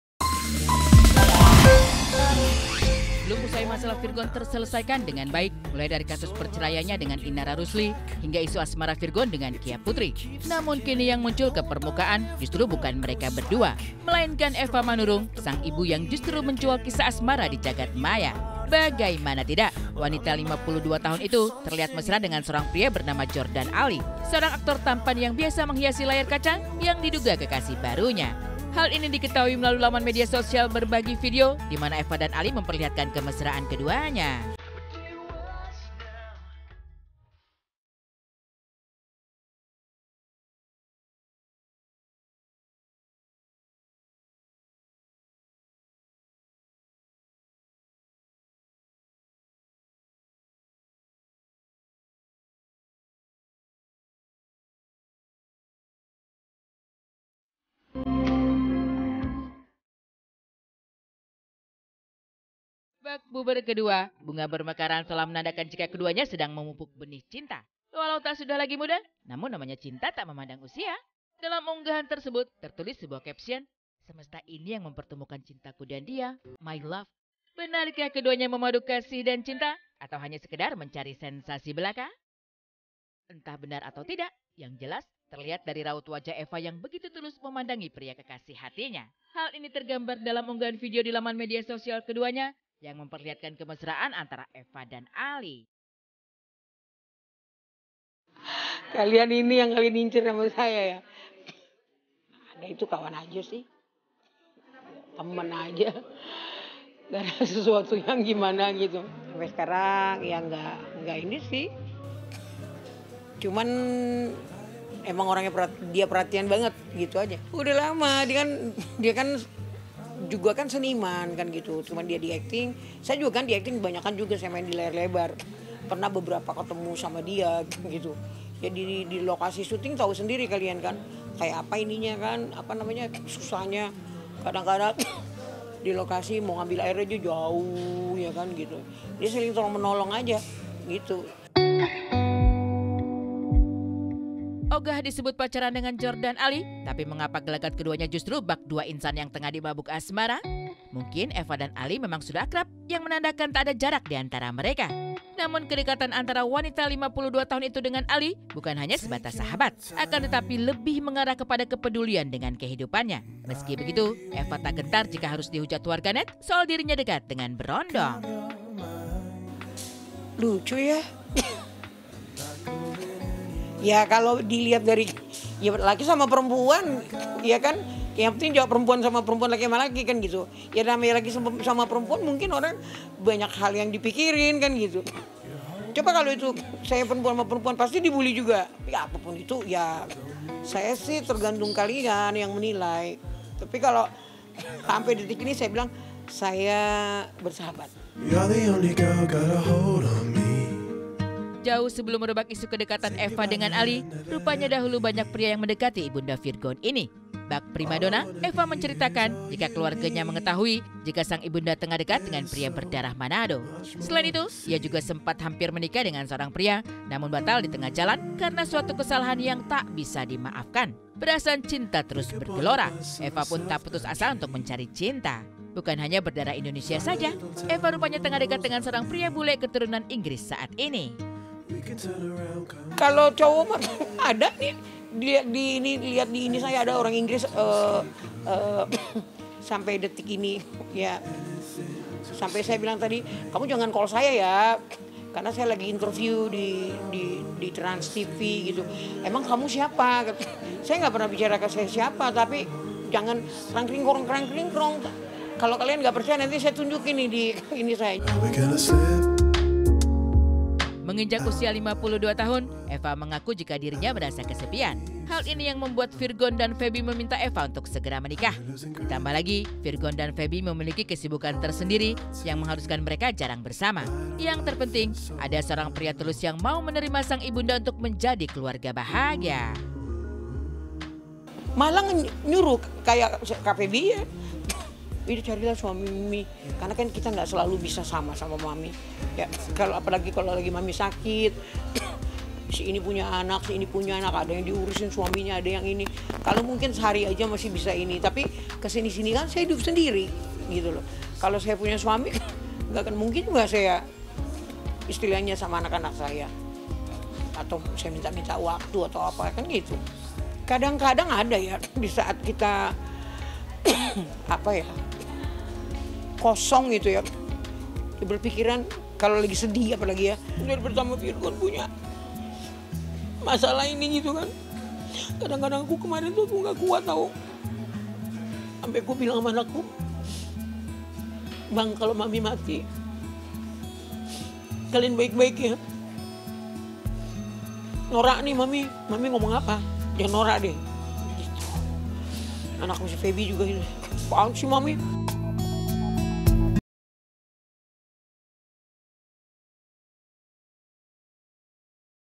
Ibunda Virgoun Puber Lagi, Katanya Lagi Deket Sama Aktor Muda Jordan Ali
Eva Manurung yang kini hidup sendiri mengaku mulai ingin memiliki pasangan lagi. Ditambah kontennya bersama Jordan Ali bikin netizen baper. Benarkah Eva kini pacari berondong?